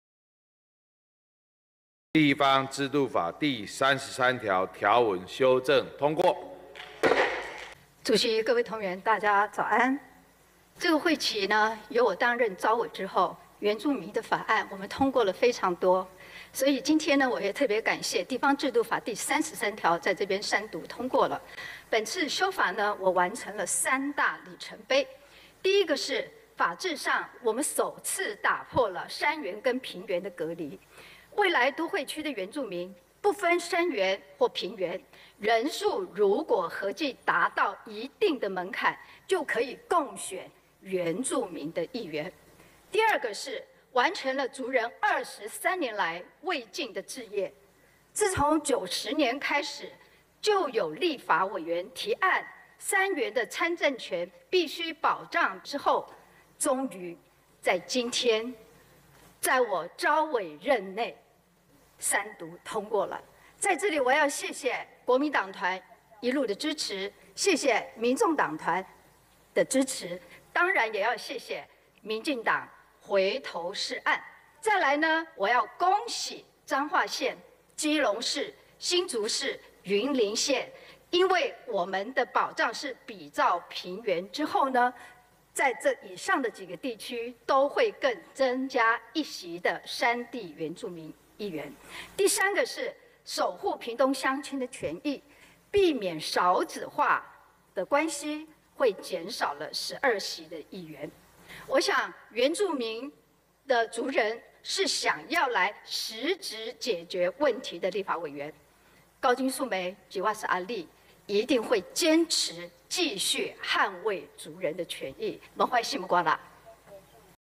《地方制度法第33條》三讀後感言~高金素梅
4月18日我在內政委員會排審《地方制度法》、5月15日及7月5日朝野協商，感謝國民黨團、民眾黨團一路以來的支持，我也要感謝民進黨團回頭是岸，最終朝野一致支持維護原民的參政權，成為立法院最美麗的風景。本次的修法，我完成了三大里程碑：一、法制上首次打破山原平原的隔離，未來都會區原住民，不分山原平原，人數合計達一定門檻，可共選原民議員。二、完成族人23年未竟的志業。自90年有立委提案山原參政權保障比照平原後，終於在今天三讀通過。也恭喜彰化縣、基隆市、新竹市及雲林縣將各增1席山原議員。三、守護屏東鄉親的權益，避免因少子化減少12席議員。原住民要的是能夠實質解決問題的立委！我，高金素梅，會繼續捍衛族人的權利！